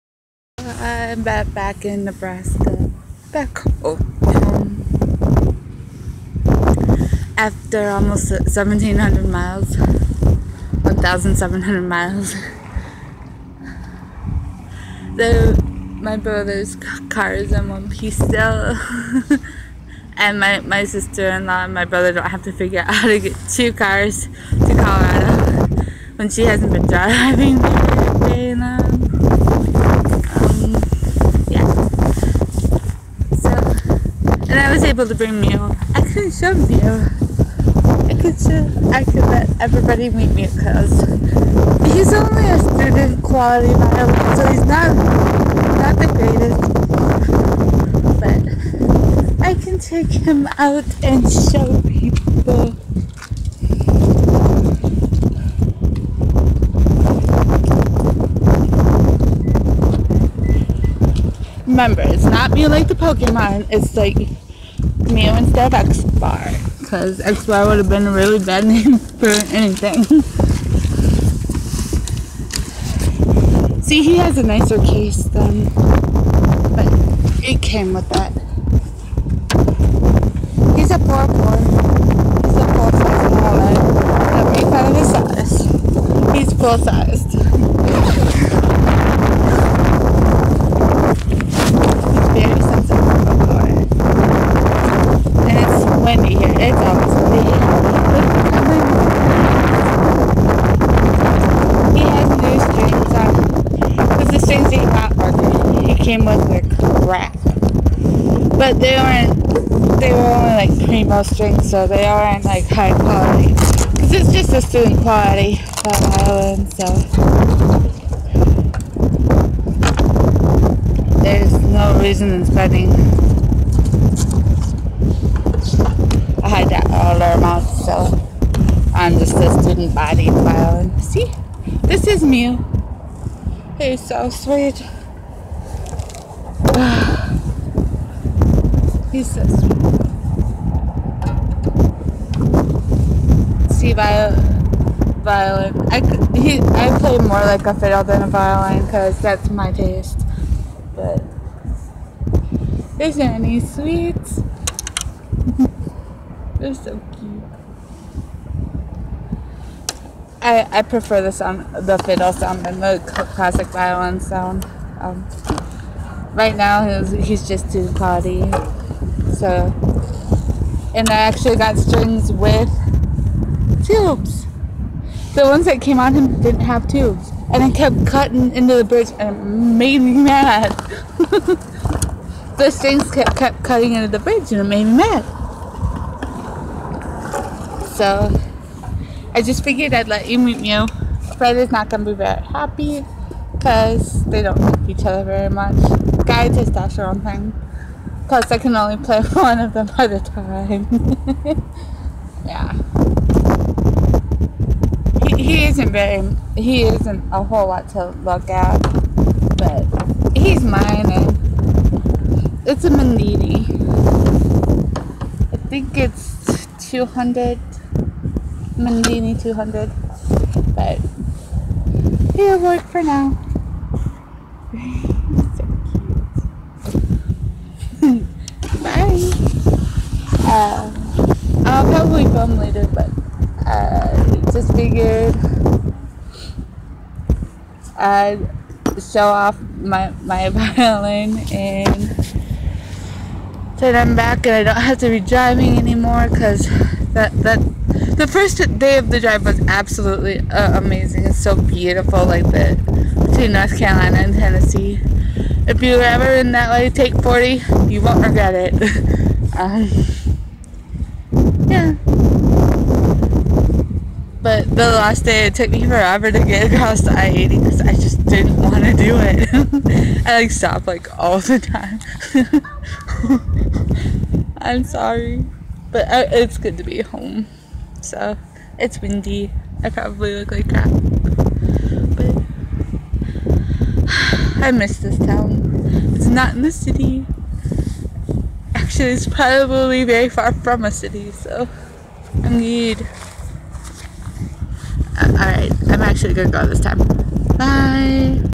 Made it to nowhere Nebraska.
Meet Mu :) (my fiddle) he wasn't named for the Pokemon.. -.-  I know he's not much to look at but I'm no Sara Watkins either.

I am glad to be back in the middle of nowhere, western Nebraska. :D